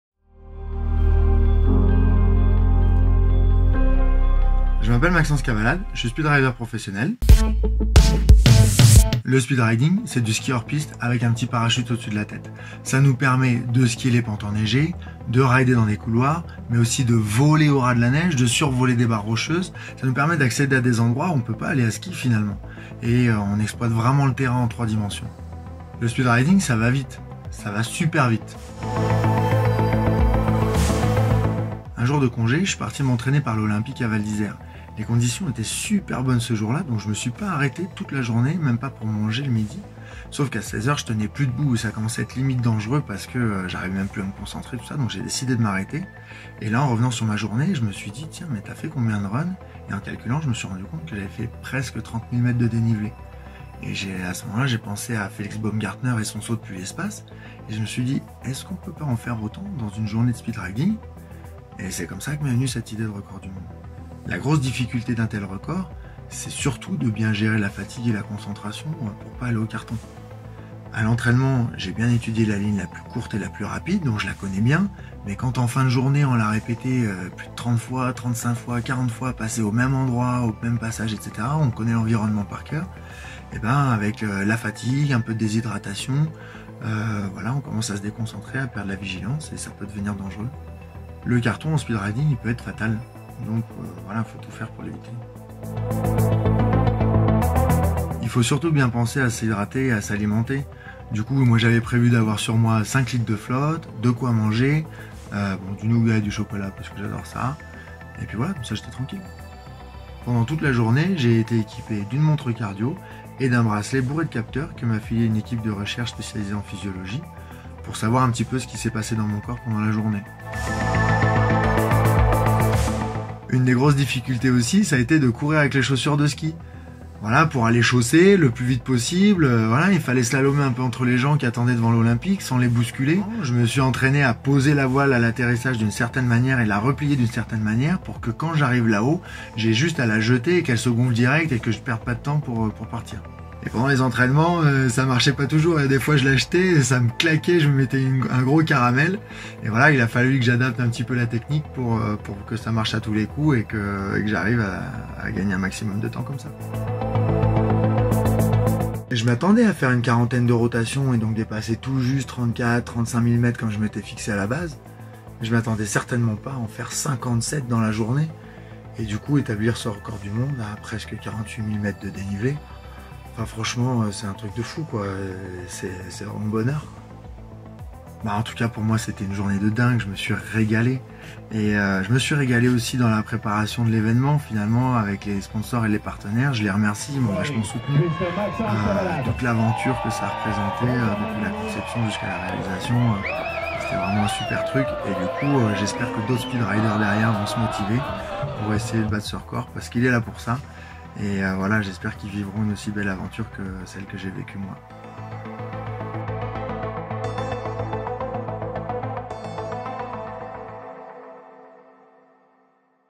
Speed riding World Record / Record du monde en Speed riding
The vidéo of Maxence Cavalade's WORLD RECORD of 47768m of vertical drop ridden in a day of speed riding in Val d'Isère.

La vidéo du RECORD DU MONDE de 47768m de dénivelé établit par Maxence Cavalade en une journée de speed riding à Val d'Isère.